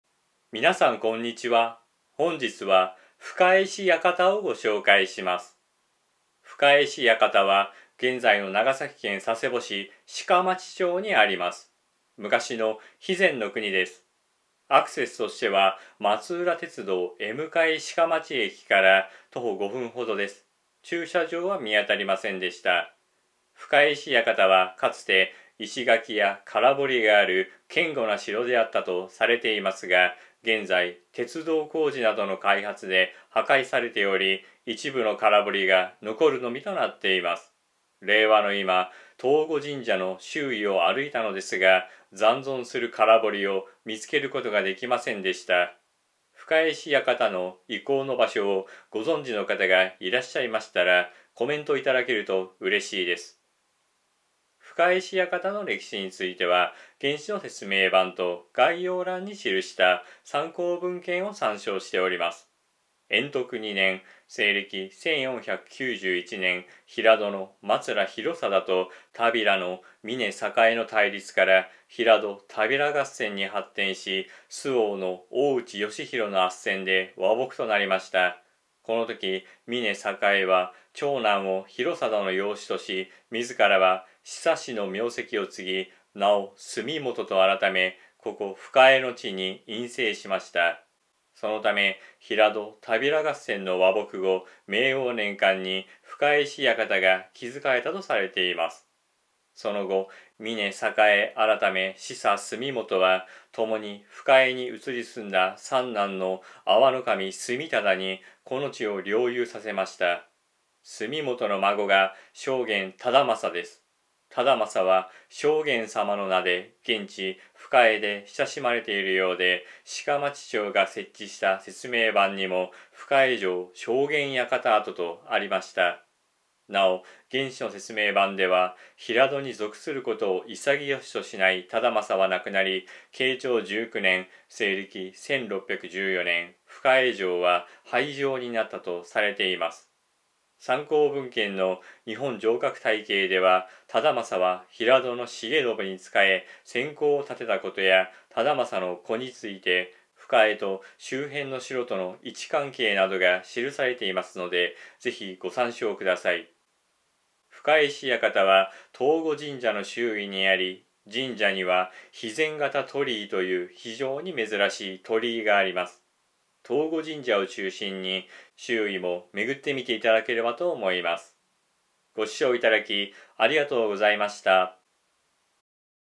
深江氏館（肥前国）【深江城】【将監さま】【藤護神社】
志佐純元（峯昌）の隠棲した地とされています。
純元の孫 忠昌は地元で将監さま として親しまれています。

【参考文献】
[1] 日本城郭体系 第17巻 長崎・佐賀、新人物往来社、昭和五十五